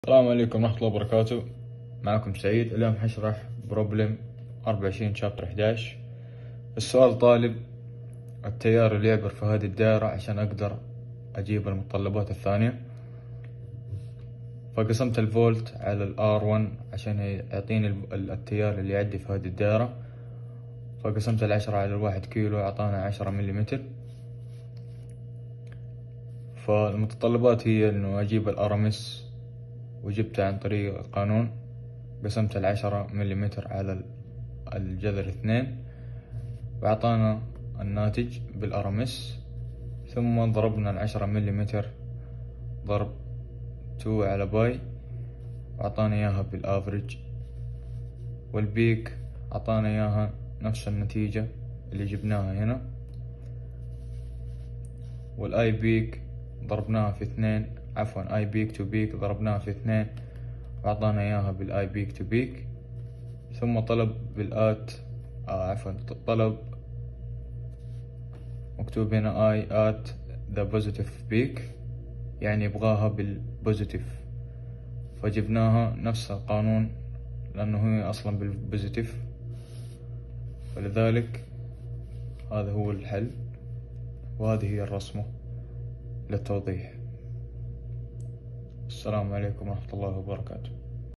sinewave parameters for current
Calculating all parameters of a current sinewave
Prepared by Saeed Baqasi
ELET102 Electric Circuits course at Electrical and Electronics Engineering Technology, YIC, Kingdom of Saudi Arabia.